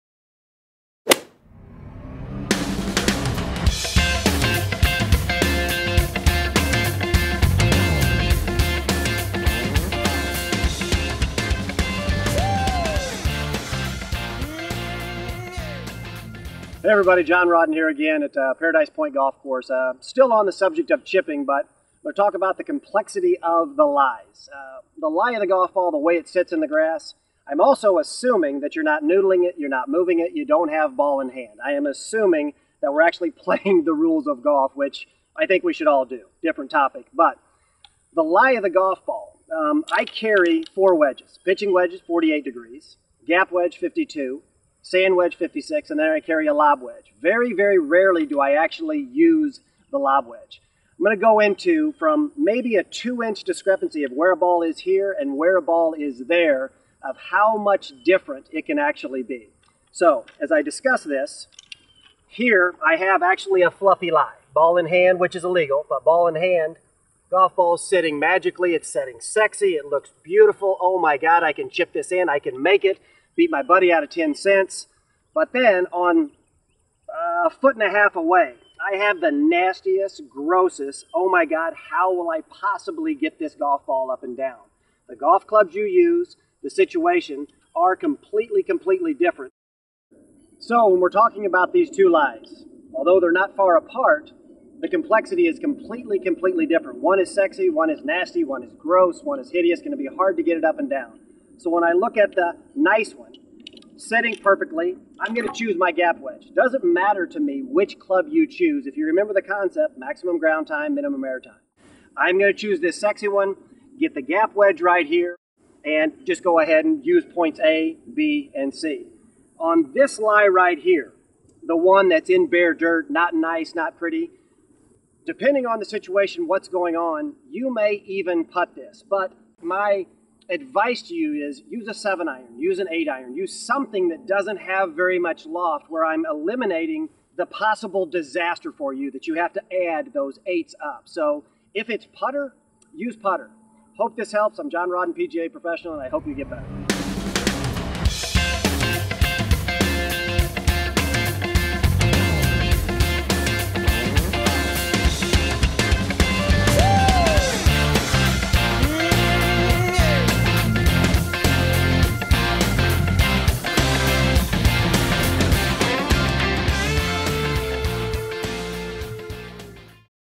John Rawdon on Chipping Lies
The club you choose to chip with often depends on the lie.
Pick the club best for that situation.